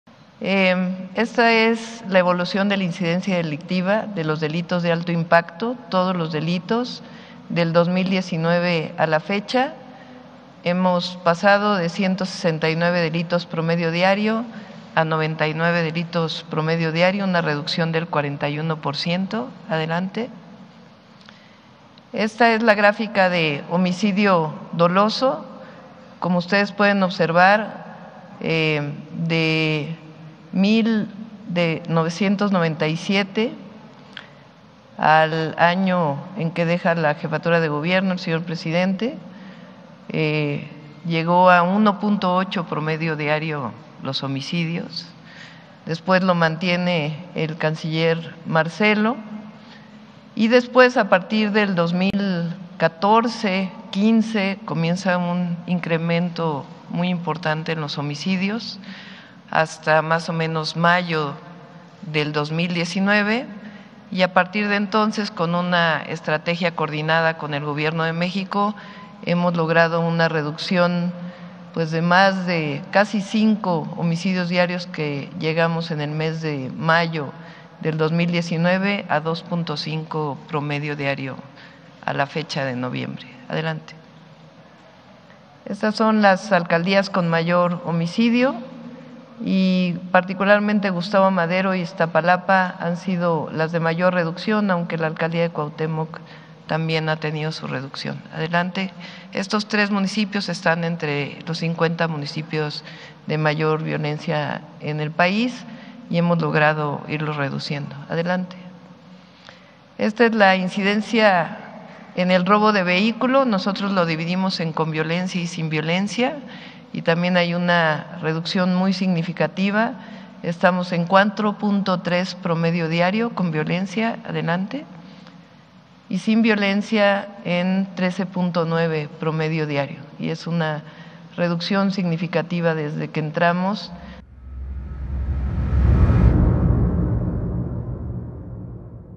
CDMX HA REDUCIDO SUS ÍNDICES DE INSEGURIDAD EN UN 41%: Sheinbaum
VIDEO La jefa de gobierno, Claudia Sheinbaum destacó la labor en coordinación con el gobierno federal para la obtención de buenos resultados